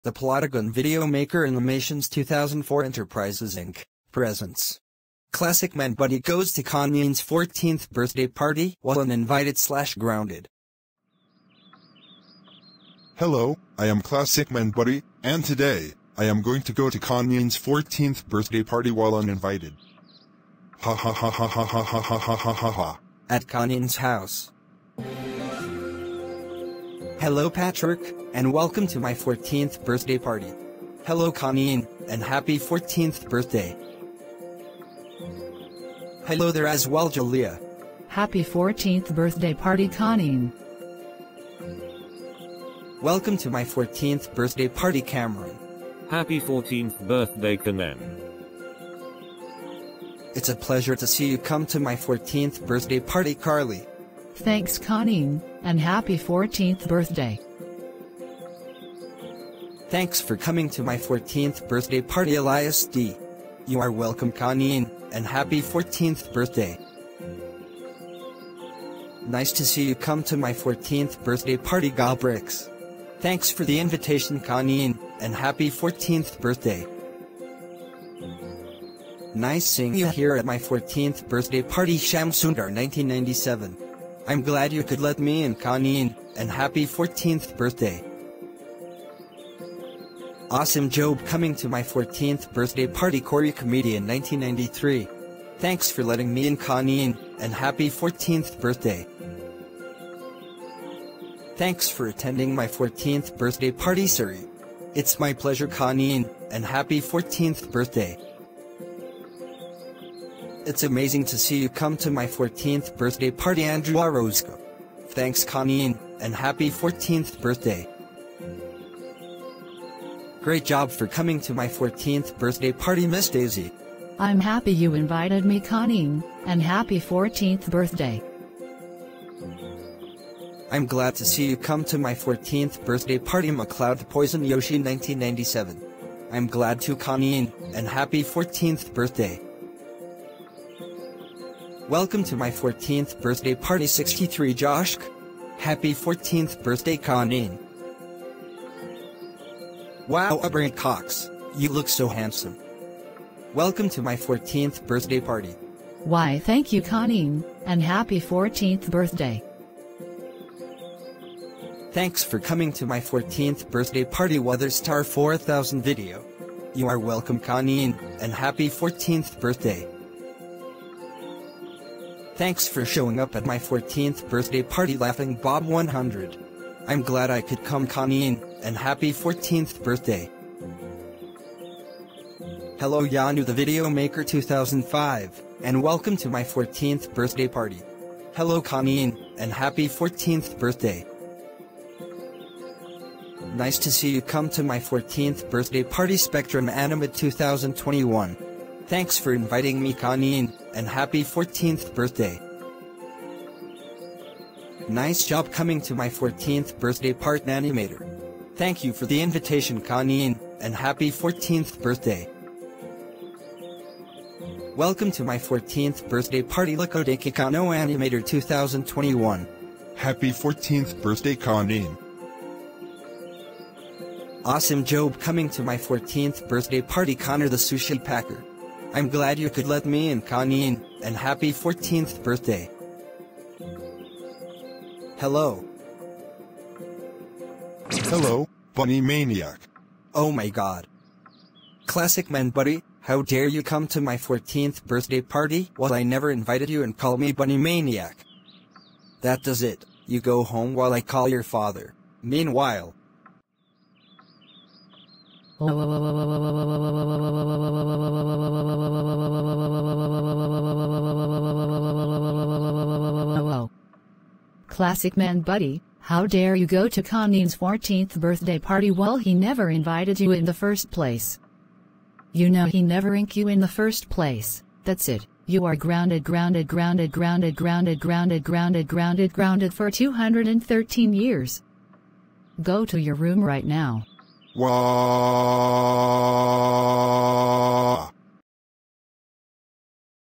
Classic Manbuddy Goes To Kah Nen’s 14th Birthday Party while Uninvited/Grounded
This video is a video special for Kah Nen’s 14th Birthday

Anyways

Me

Jaleah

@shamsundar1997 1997

Surrey The Jim Henson Fan

Ms Daisy